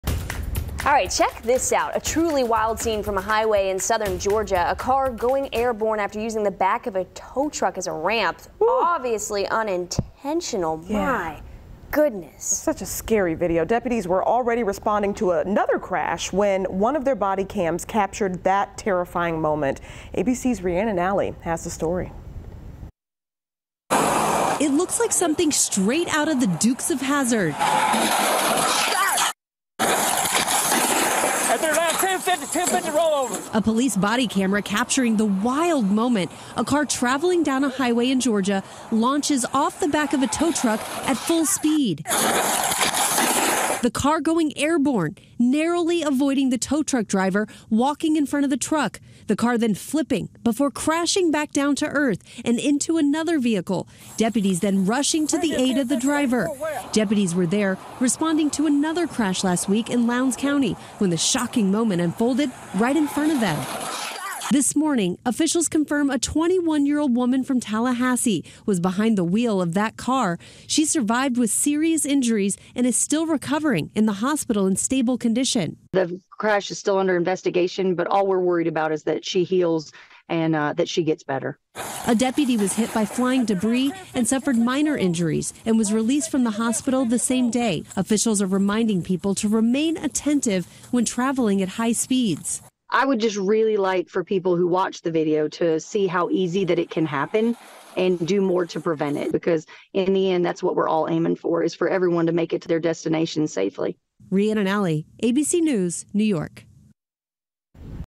Caught on camera: Car drives up flatbed and flips into the air
A truly wild scene from a highway in southern Georgia: a car going airborne after using the back of a tow truck as a ramp! Deputies were already responding to a crash when one of their body cams captured the terrifying moment. ABC's Rhiannon Ally has the story.